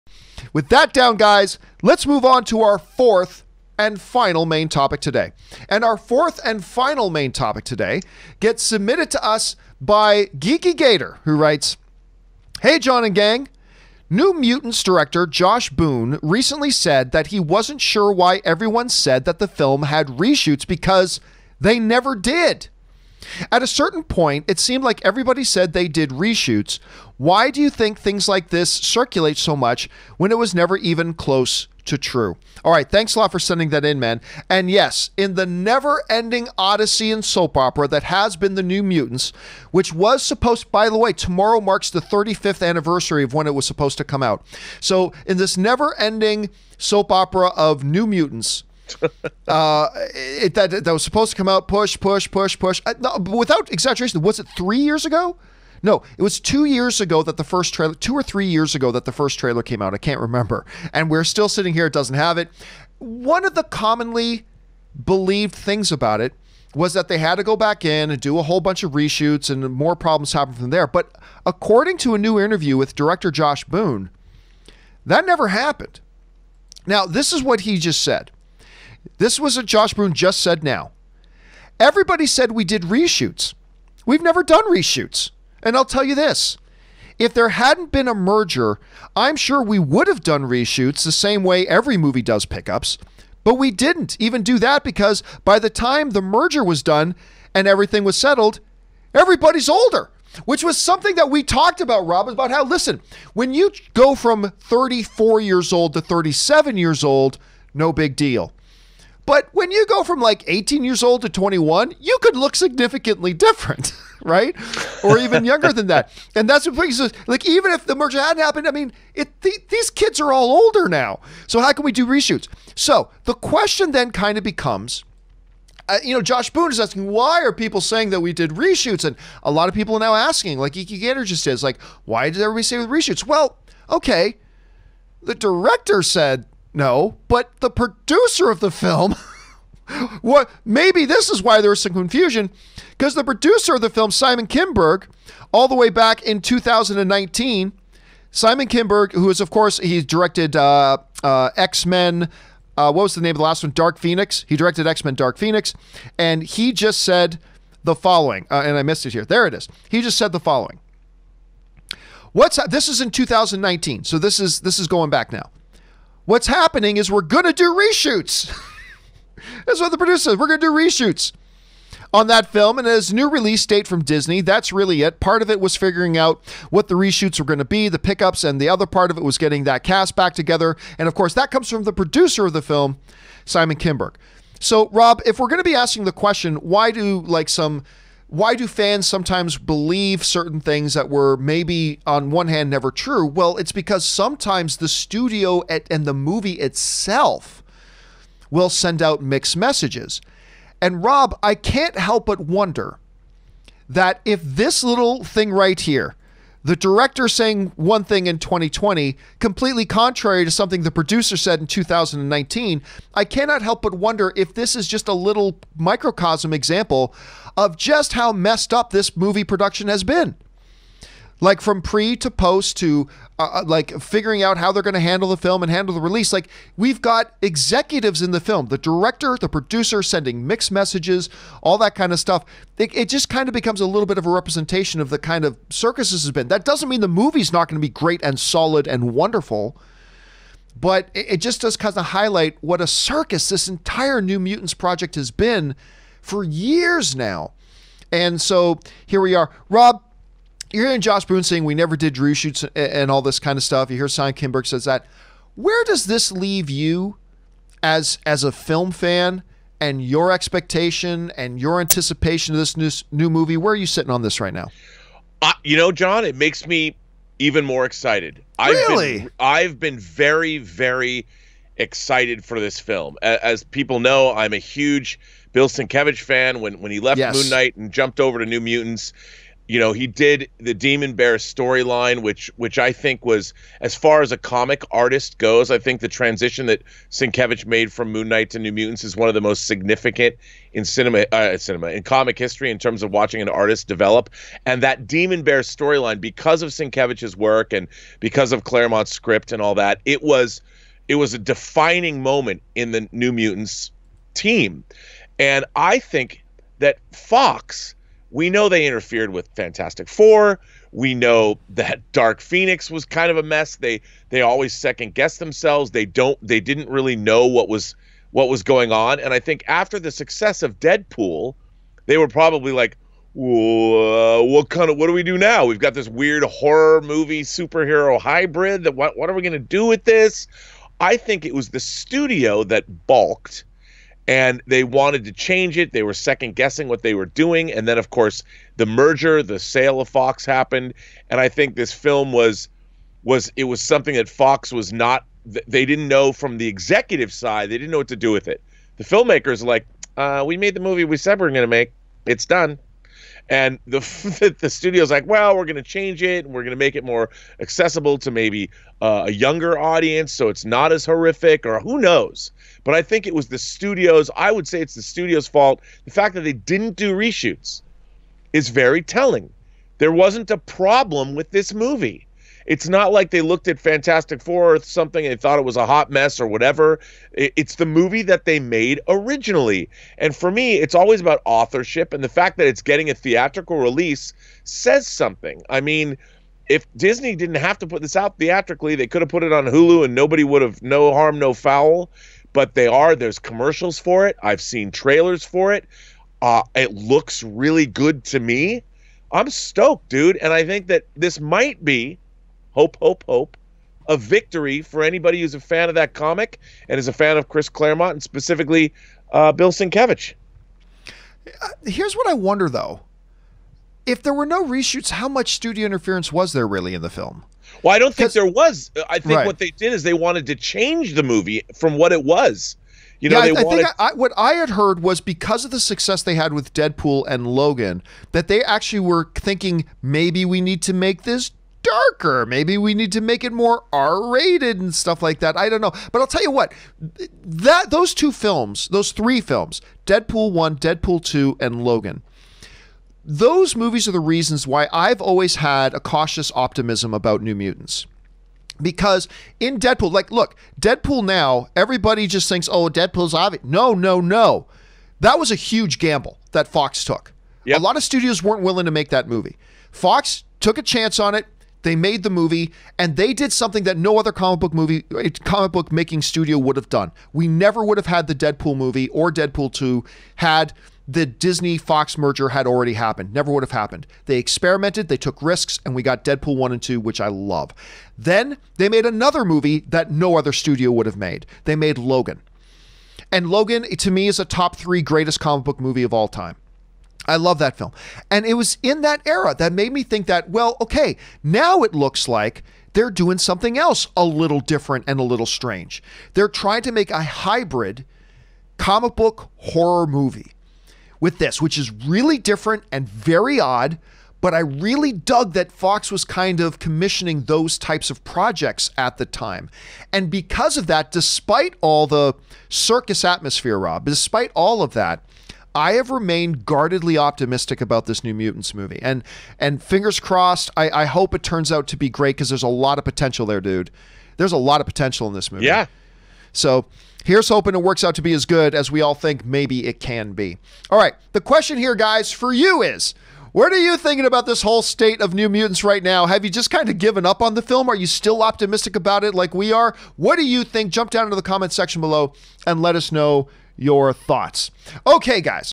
Director Says No New Mutants Reshoots Happened - But Producer Said There Were
Geeky Gator - Hey John and gang! New Mutants Director Josh Boone recently said that he wasn’t sure why everyone said the film had reshoots because they never did. At a certain point it seemed like everyone said they did reshoots. Why do you think things like this circulate so much when it was never even close to true?